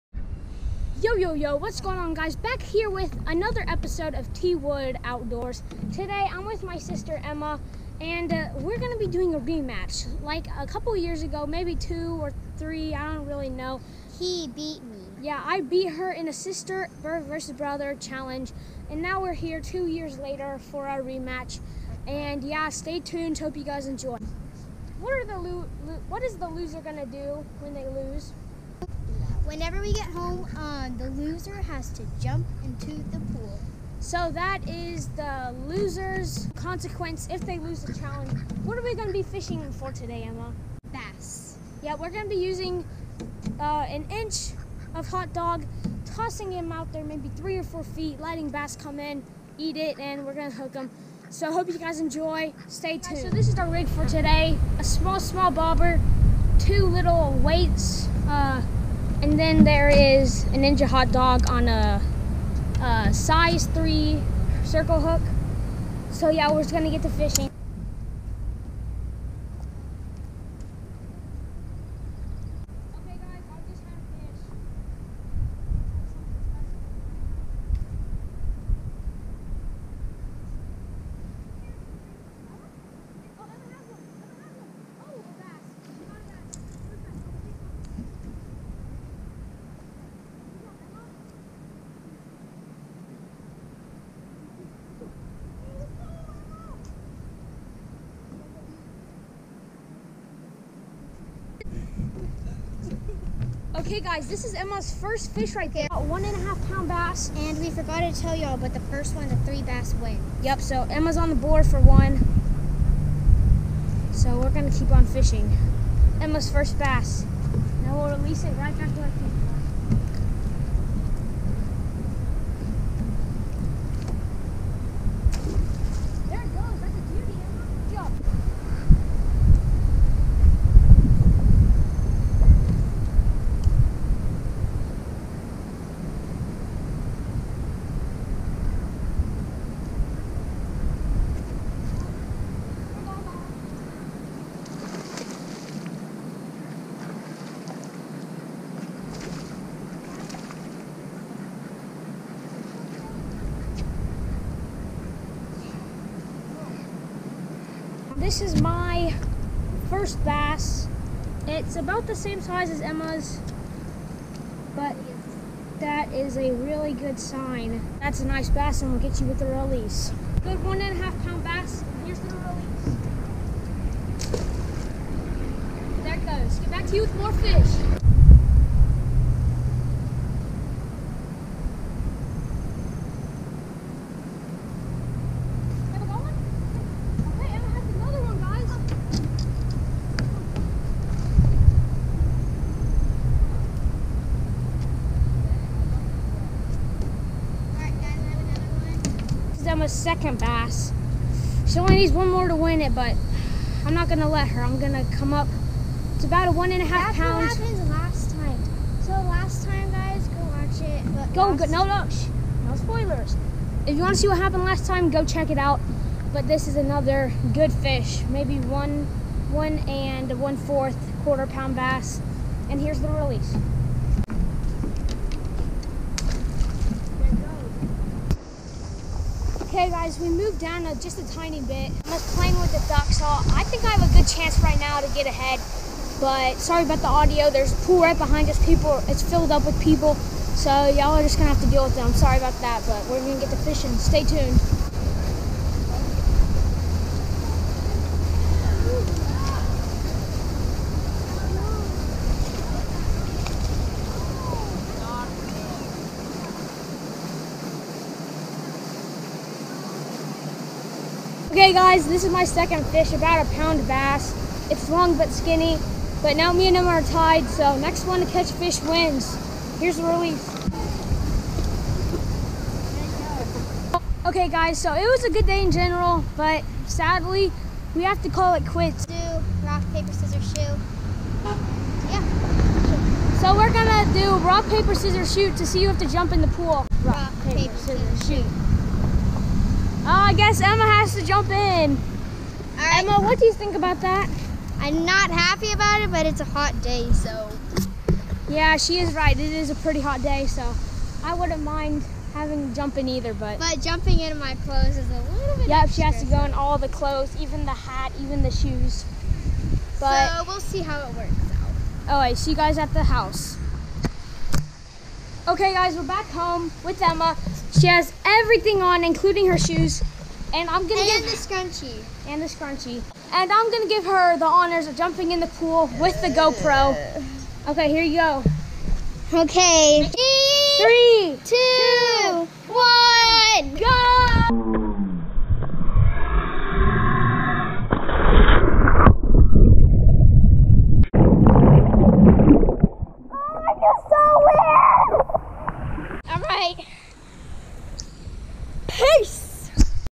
Sister VS Brother Fishing Challenge!(Rematch | Loser Has to...)
Hope you guys enjoyed! If you have a sister or brother you should do this with them. It was a fun and good day on the water. And like always check out Myfinfinder and Myfinfinder.com!